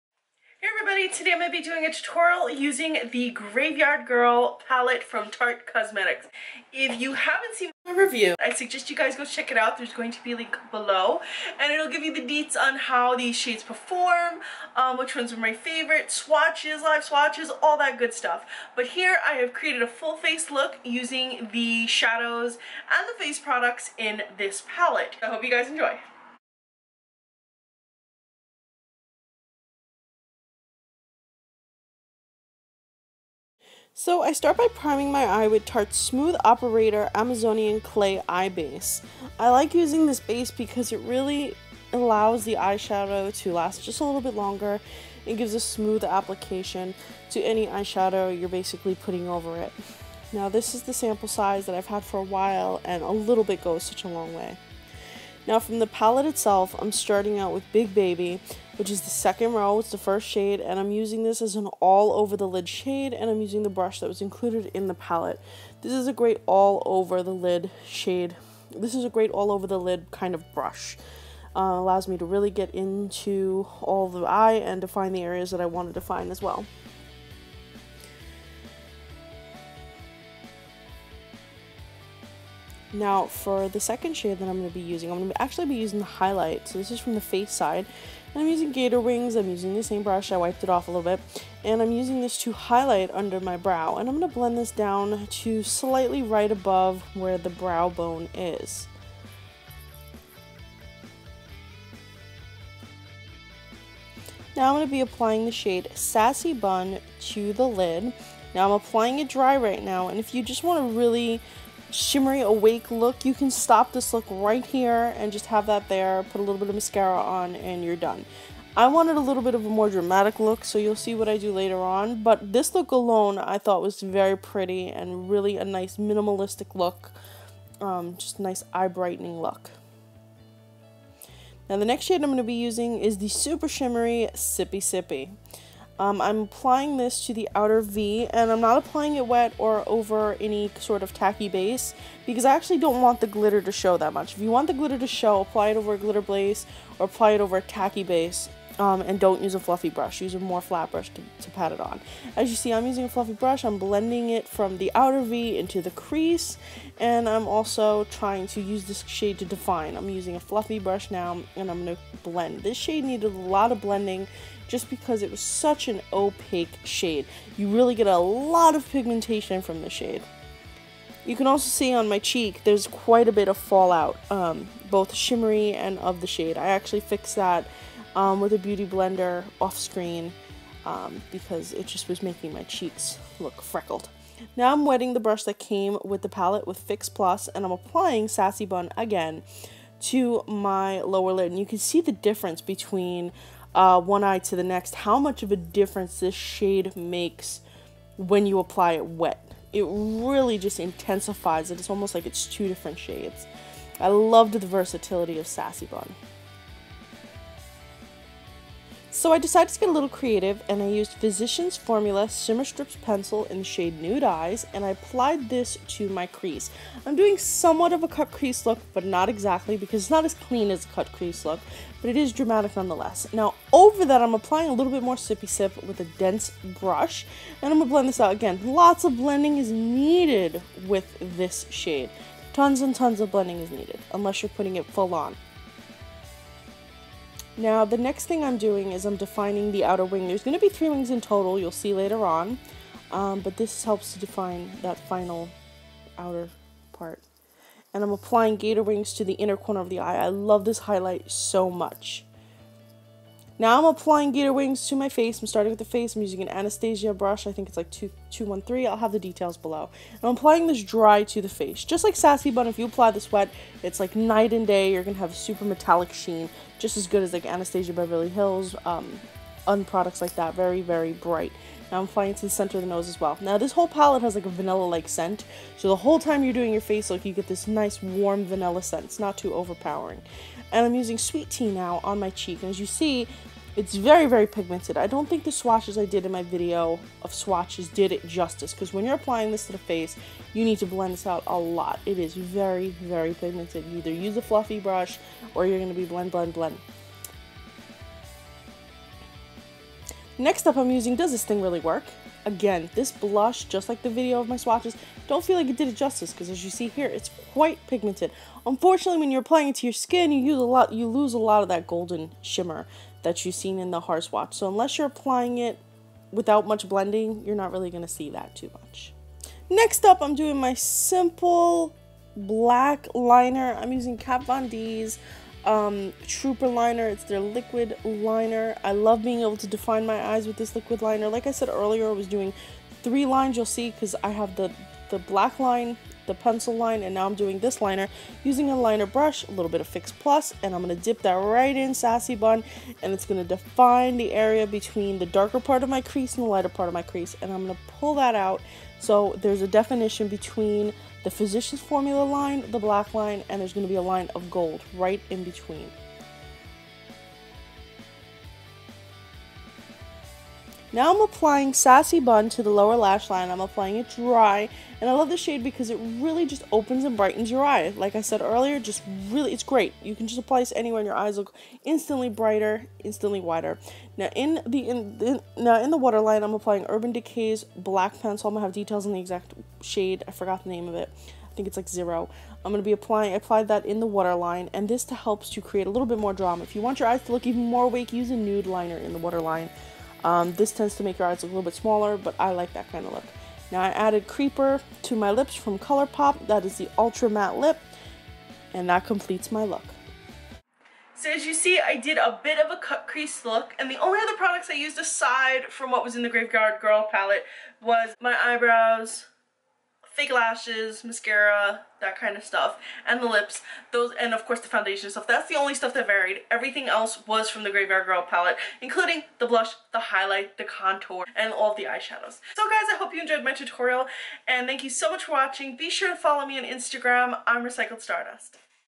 Tarte Grav3yardgirl Swamp Queen Palette Tutorial
The new limited edition Tarte Cosmetics Grav3yard Girl Swamp Queen palette is the perfect way to switch up your everyday makeup routine.

Shades from the palette:
Big baby all over the lid
Sassy bun both wet and dry on the lid. I also used this shade on the lower lash line.
Sippy Sippy on the crease and outer v
Gator wings on the inner corer on the eye and under the brow to highlight
Gator wings to highlight the cheeks and nose
Does this thing really work- blush on cheeks
Sweet tea to contour

Products I used:
Primer- Lancôme la base pro
Foundation- Tarte rainforest of the sea light-medium neutral
Concealer- Tarte rainforest of the sea light-medium
Setting powder- too faced primed and poreless
Liner pencil for crease- physicians formula shimmer strips in the shade nude eyes
Liner for water line- urban decay glide on eye pencil in the shade zero
Eye primer- Tarte smooth operator
Lips- Colourpop ultra matte lip in the shade creeper
Brows- Colourpop bangin brunette brow liner